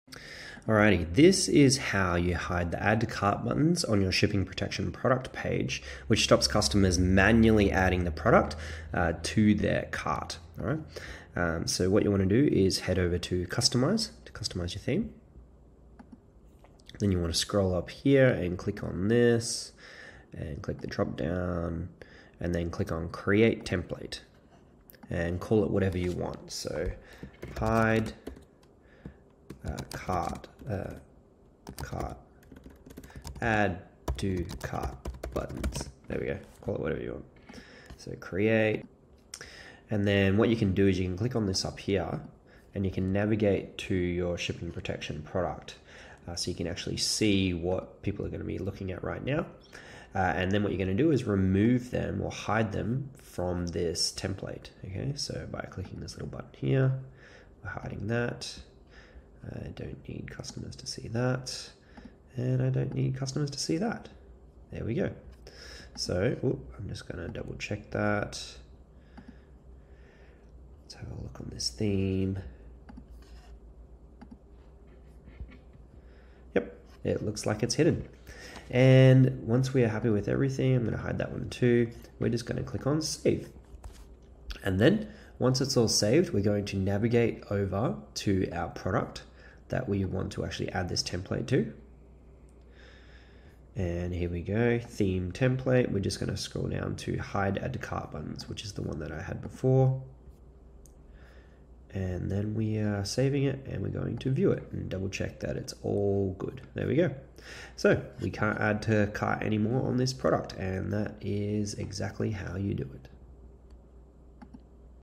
Hide add to cart button on product page Shopify | Shipping Protection Tutorial
Learn how to hide add to cart on specific product pages using theme product templates. This is especially useful for Shipping Protection product types.